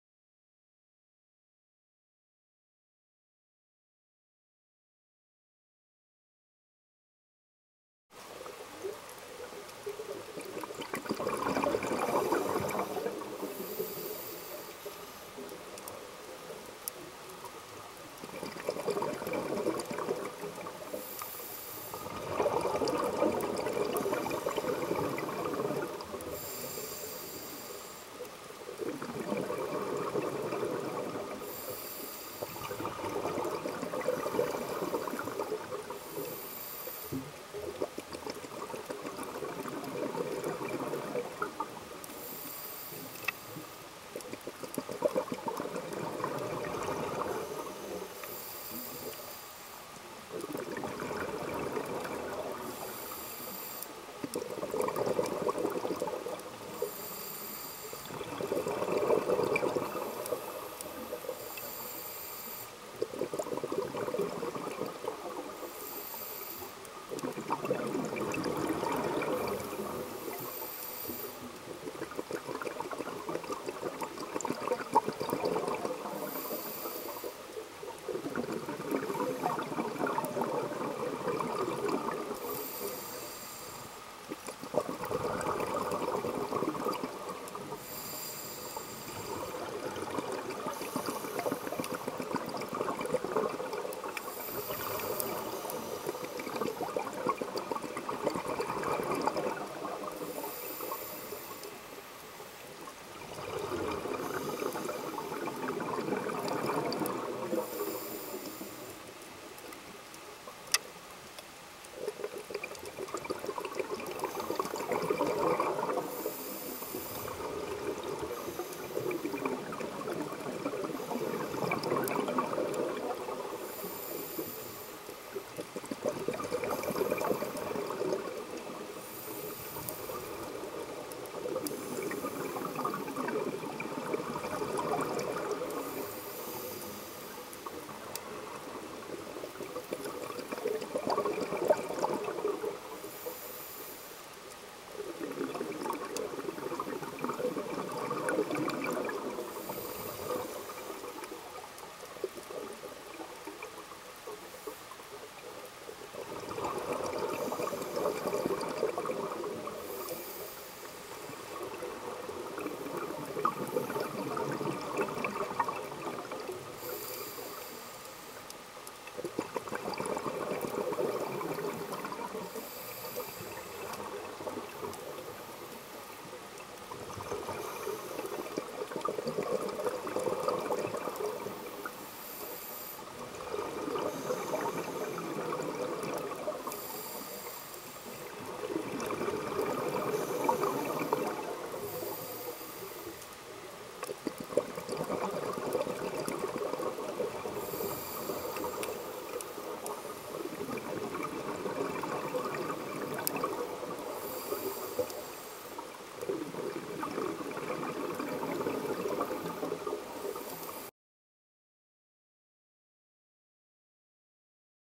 Mimic Octopus
It's a good start of the year! I dive in Gorontalo and found this cute mimic octopus ^^ *grin* :D